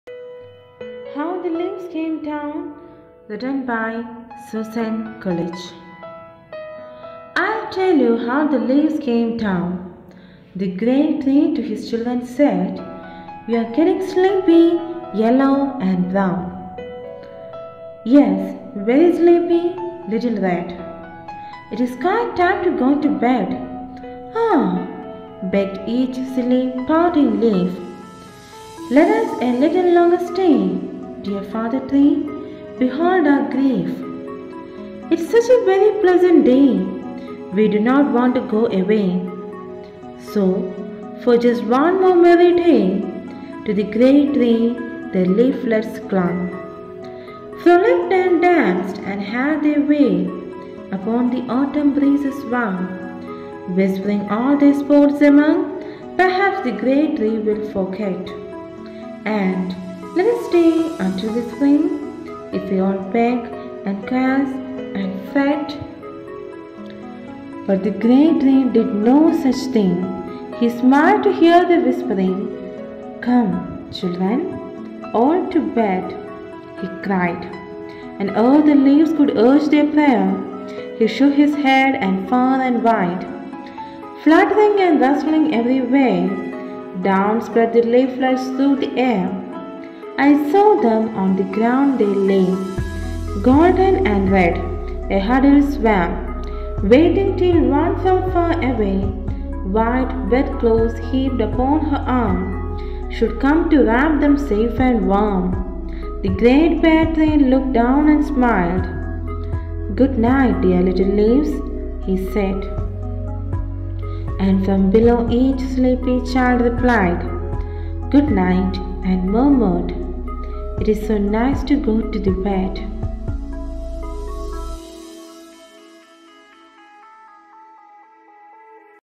How the Leaves Came Down 🍃 Susan Coolidge
Do you know why leaves fall in autumn?
Listen to this poem by Susan Coolidge to find out.